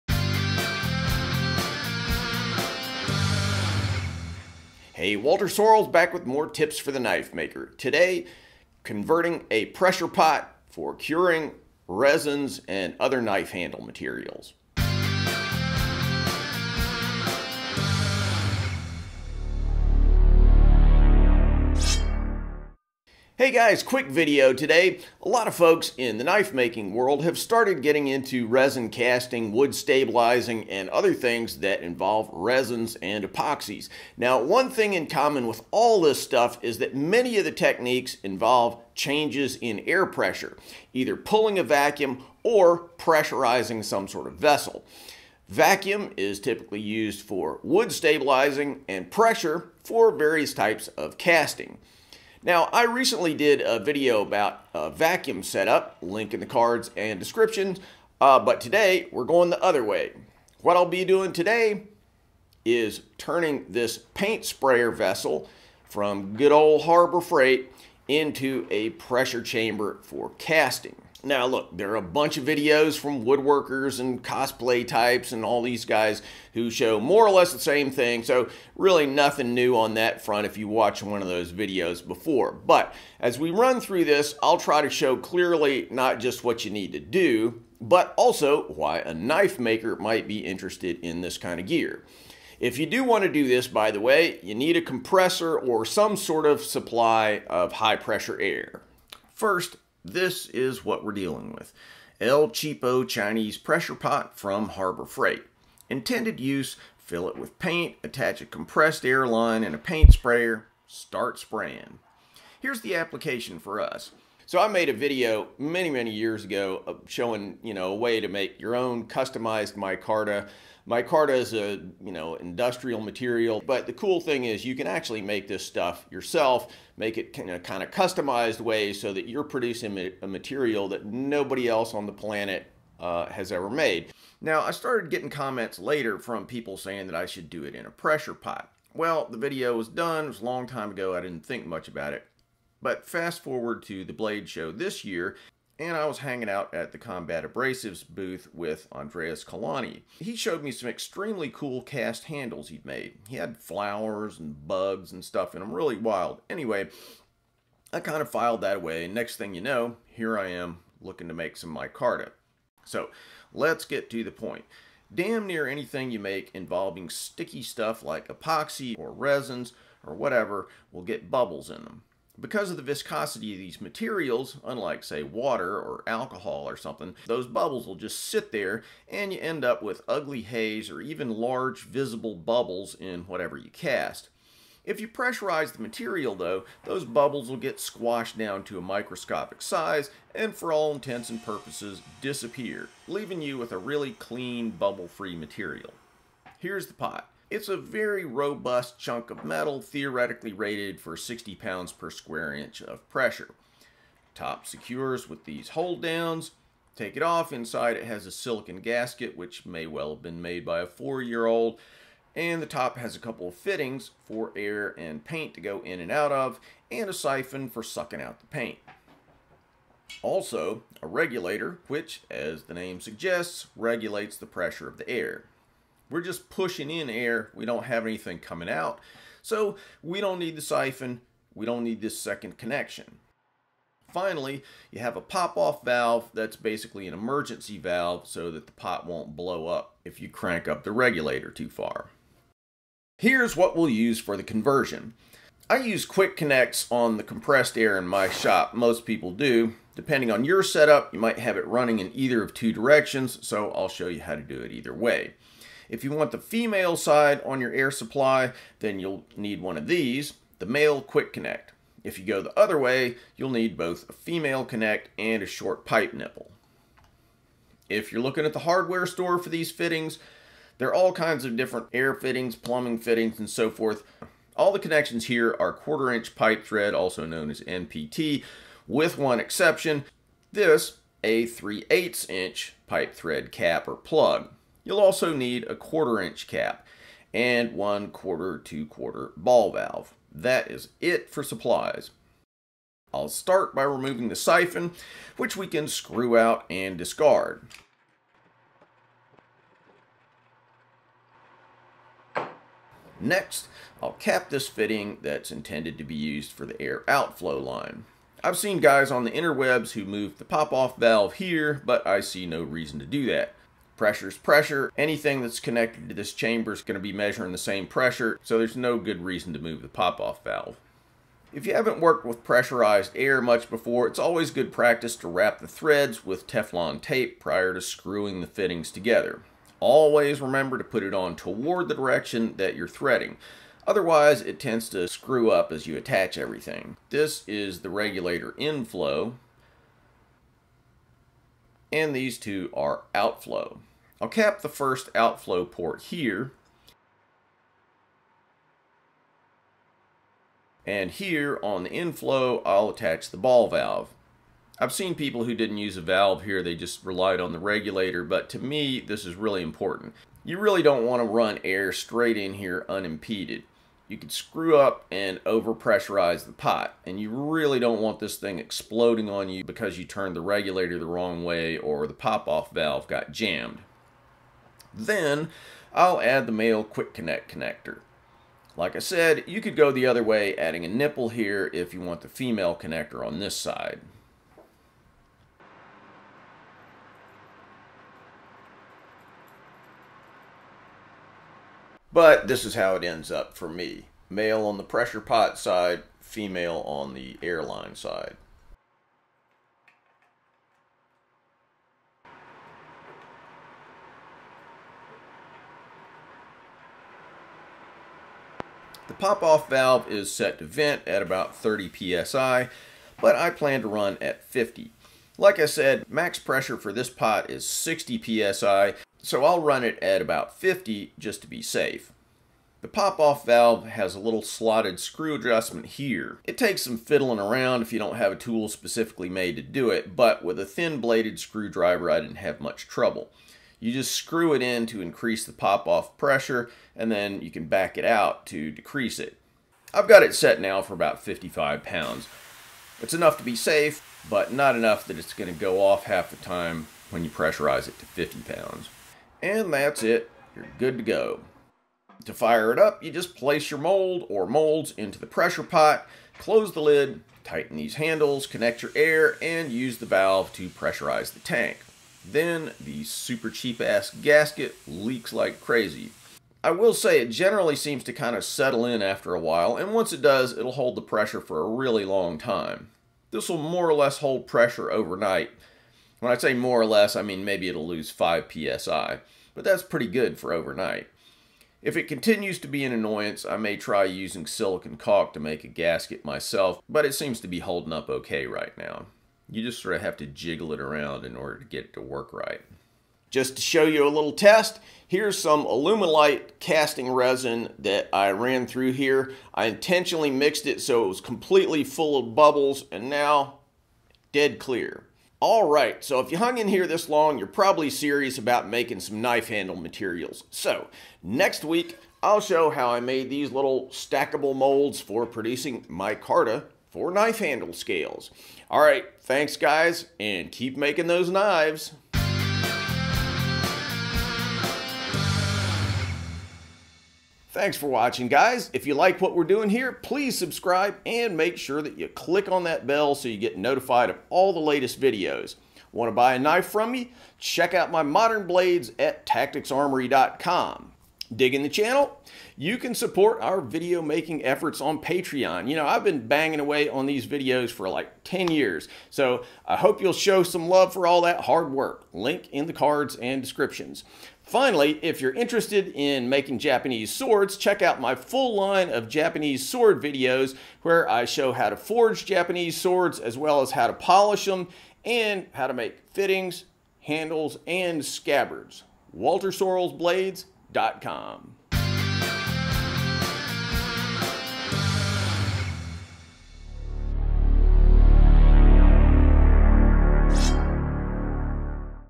Making a Cheap Pressure Pot for Knife Making Material Casting
Casting resins and epoxies have become popular with a wide variety of craftsmen including knife makers and woodworkers as well as prop and costume makers.  In this video, bladesmith Walter Sorrells shows how he turned a cheap pressure pot for painting into a pressure vessel for casting resins and epoxies.

If you'd like to see more of Andreas Kalani's knives like the one shown in the video at 2:31, check out

Walter's Instagram:  walterstactix
Tactix Armory Instagram:  tactixarmory

Help the channel and get a discount on belts from Combat Abrasives!